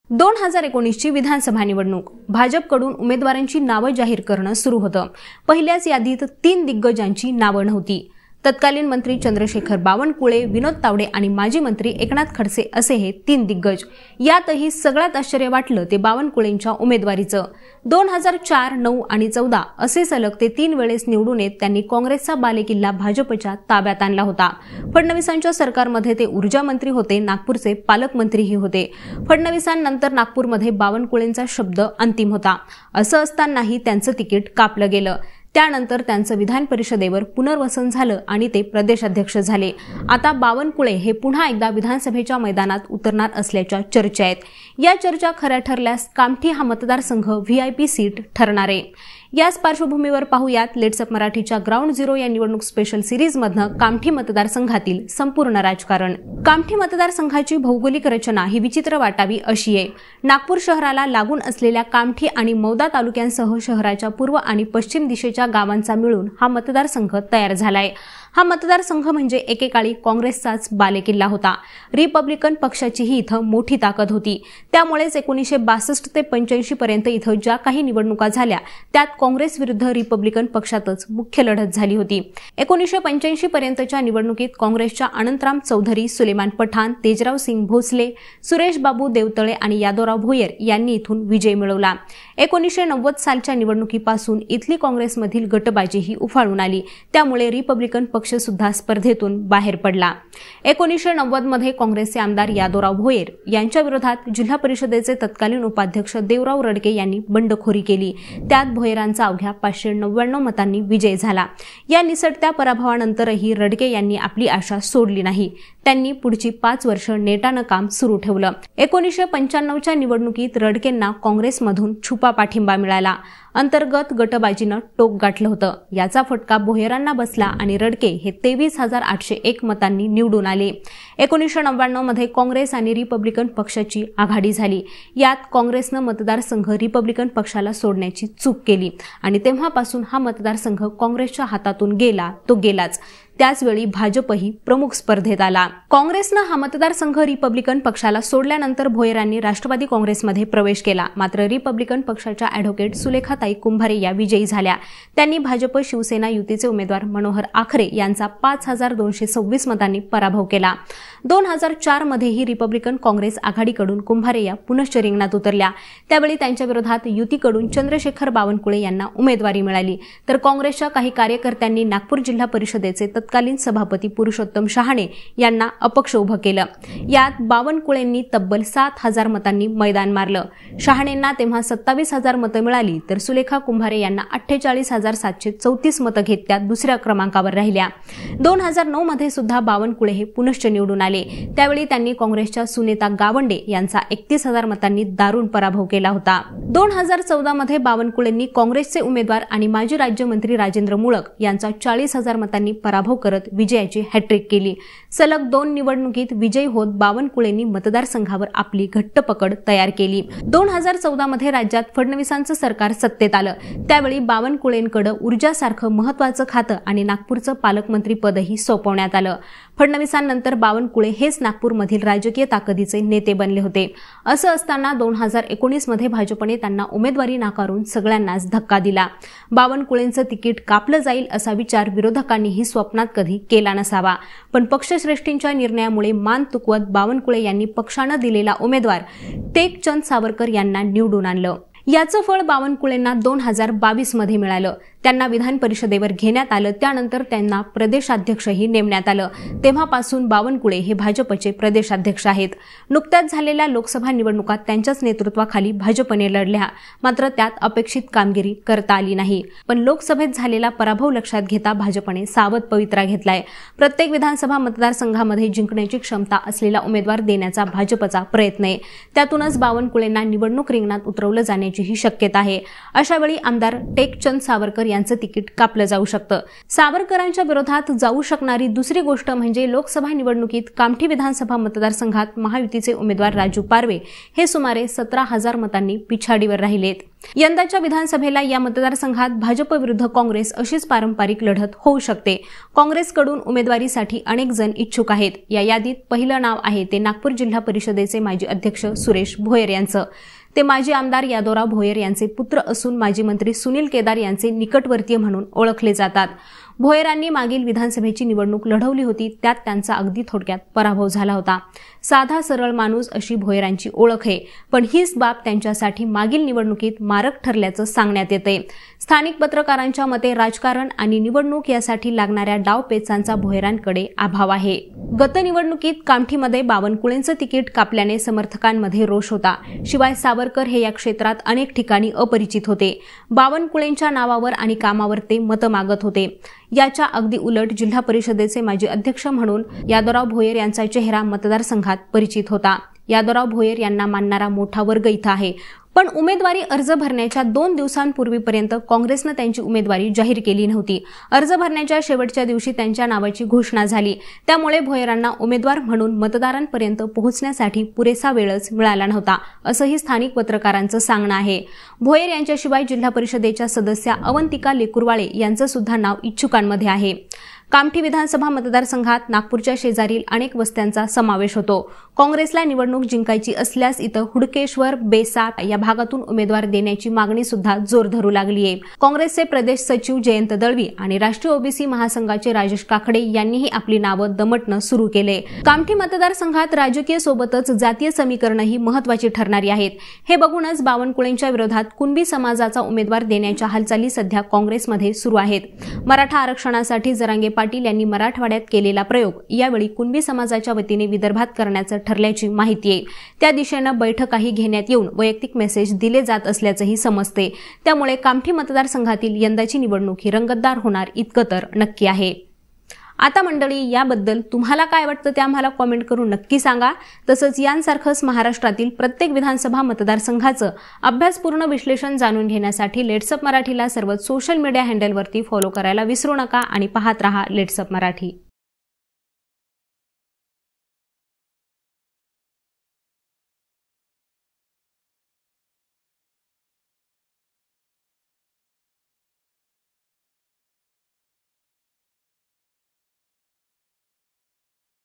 Kamathi Vidhansabha : कामठीची सीट व्हिआयपी ठरणार; बावनकुळे पुन्हा मैदानात उतरणार | LetsUpp Marathi
भाजपचे प्रदेशाध्यक्ष चंद्रशेखर बावनकुळे हे पुन्हा कामठी या मतदारसंघातुन मैदानात उतरणार असल्याच्या चर्चा आहेत. त्यामुळे कामठी मतदारसंघ व्हिआयपी सीट ठरणार आहे. नेमक काय आहे कामठी मतदारसंघातील संपूर्ण राजकारण? याचाचं लेट्सअप मराठीने घेतलेला ग्राउंड झिरो आढावा...